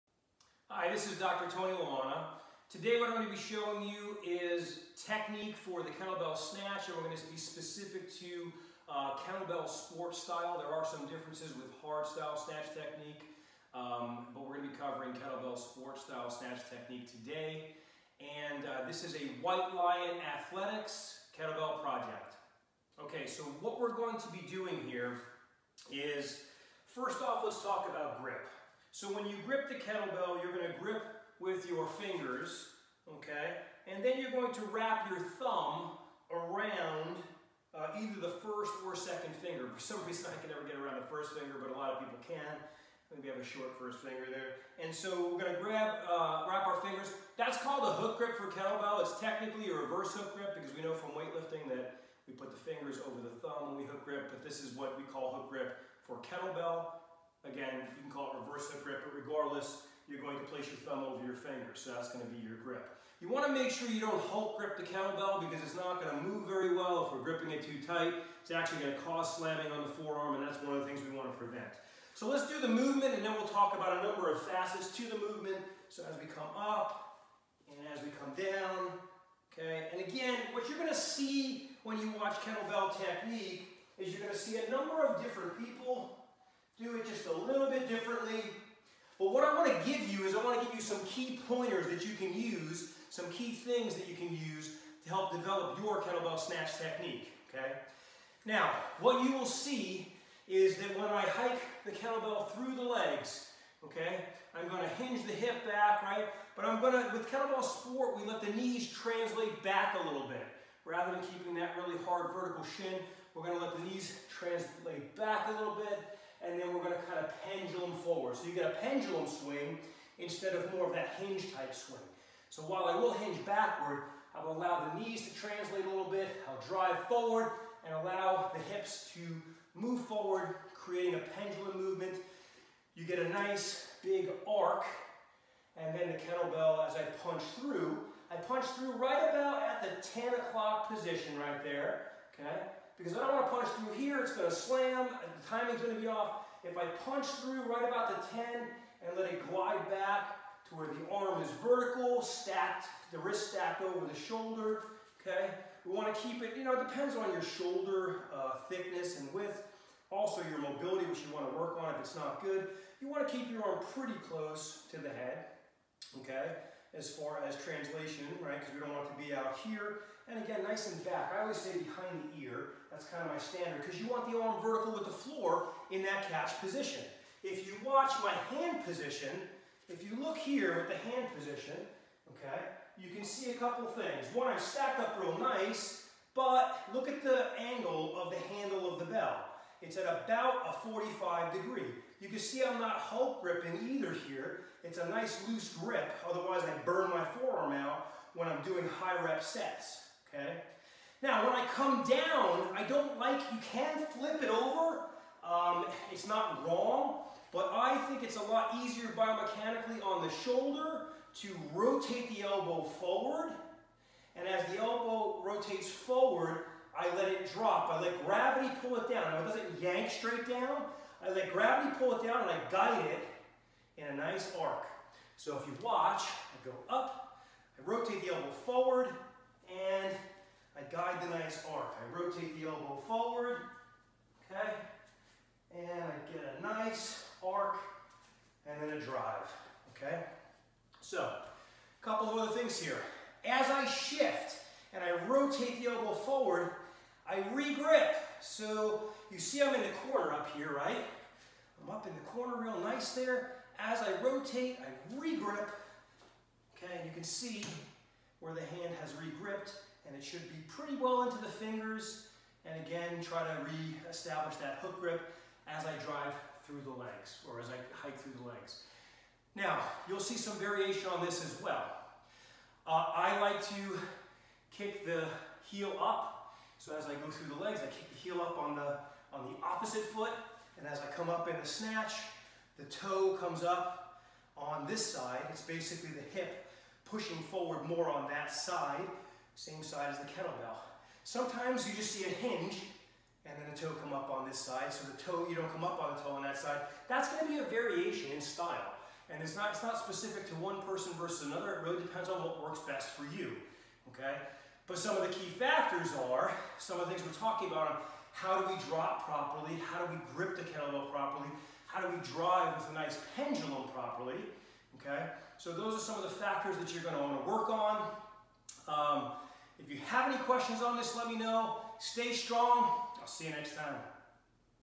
How to do the Kettlebell Snatch| Single Arm Kettlebell Snatch Technique
Shipping WorldWide
How to Perform you Kettlebell Snatch.
This is the second part in a two part series with IKLF  2018 Gold and Silver Medalist (USA NATIONALS)  and IKLF 2018 Silver Medalist (World Championships) Dr. Tony Lamanna.

The kettlebell snatch is clearly a full body, ballistic movement that requires shoulder mobility, stability and strength. Additionally, there needs to a sufficient amount experienced with single arm kettlebell swings and exhibiting core strength during your kettlebell swing as well in transition to the overhead position.

Watching a single tutorial on Kettlebell Snatches will not be sufficient for safely and consistently performing kettlebell snatches with any load.  The Kettlebell Snatch should be approach be with caution - as with any ballistic movement.

Dr. Lamanna's approach to training is very technical yet his explanations are very easy to understand because he explains difficult concepts with ease. We would encourage you to visit his Instagram, YouTube and Facebook pages where he presents a lot of his training but also provides very conscise explanations on his approach and methods.

We are very happy that we have the privilege of Dr. Tony Lamanna agreeing to create a very thorough and thoughtful tutorial on for a kettlebell snatch.

Dr. Lamman is a Board Certified in Chiropractic and Physiotherapy and also holds a Masters in Human Nutrition. Dr. Lamanna is also Level 1 & 2 USA Weightlifting Sports Performance Coach; Mace Training Instructor (through Mr. Maceman Rik Brown); Corrective Exercise Specialist (NASM); and USA Powerlifting Club coach.